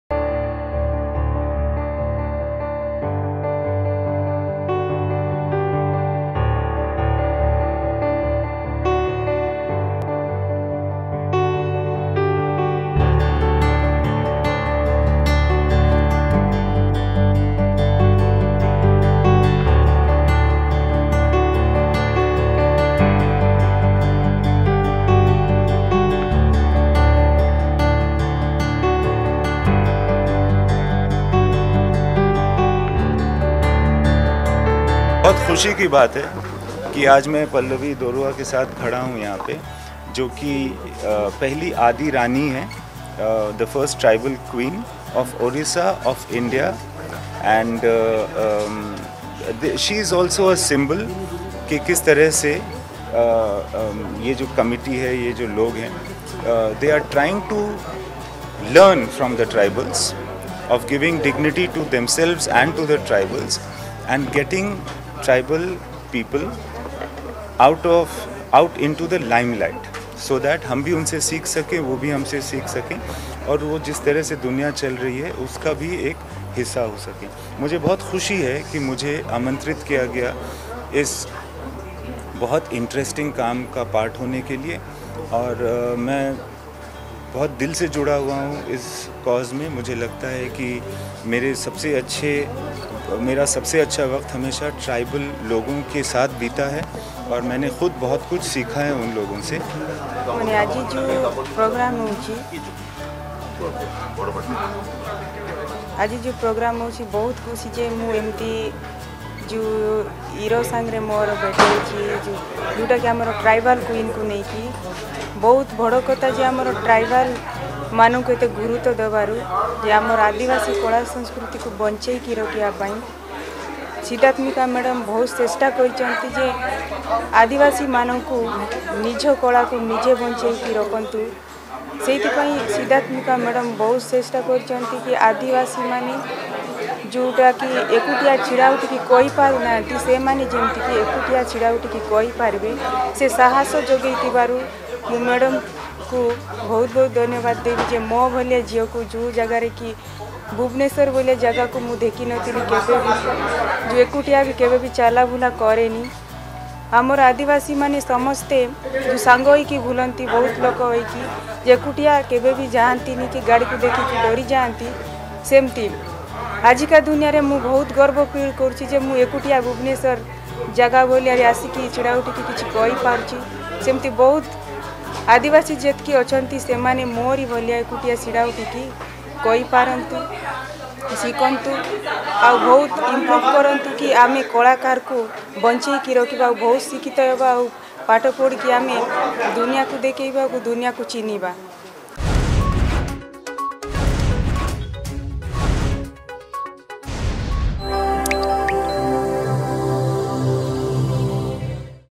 ଟ୍ରାଇବାଲ କୁଇନ୍ ଗ୍ଲୋବାଲ ପ୍ରତିଯୋଗିତା ୨୦୨୨
ଭାରତରେ ପ୍ରଥମ କରି ଆୟୋଜିତ ହେବାକୁ ଯାଉଛି ଟ୍ରାଇବାଲ କୁଇନ୍ ଗ୍ଲୋବାଲ ପ୍ରତିଯୋଗିତା ୨୦୨୨ । ଏହି ଅବସରରେ ଟ୍ରାଇବଲ୍ ଫାଉଣ୍ଡେସନ ପକ୍ଷରୁ ଭୁବନେଶ୍ୱର କ୍ଲବ୍ ଠାରେ ଉନ୍ମୋଚିତ ହେଲା ଟ୍ରାଇବାଲ କୁଇନ ପ୍ରତିଯୋଗିତାର ମାସ୍କଟ ଓ ପୁସ୍ତିକା